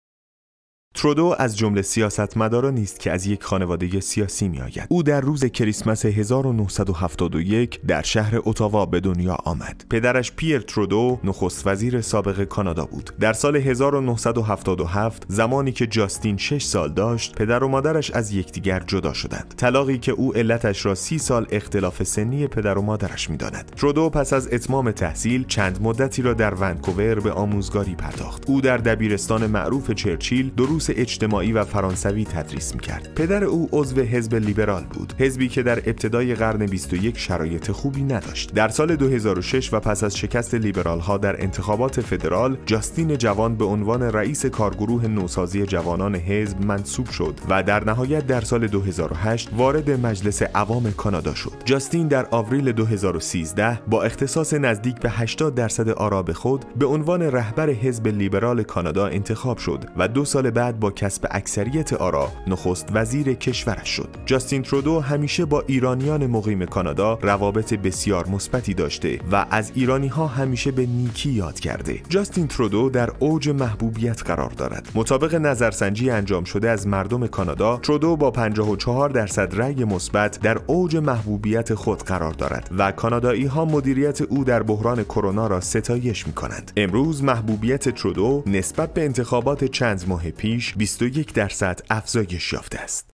دلیل محبوبیت روزافزون جاستین ترودو چیست؟
در این ویدیوی کوتاه با زندگی و دستاوردهای نخست وزیر محبوب کانادا، جاستین ترودو آشنا می شویم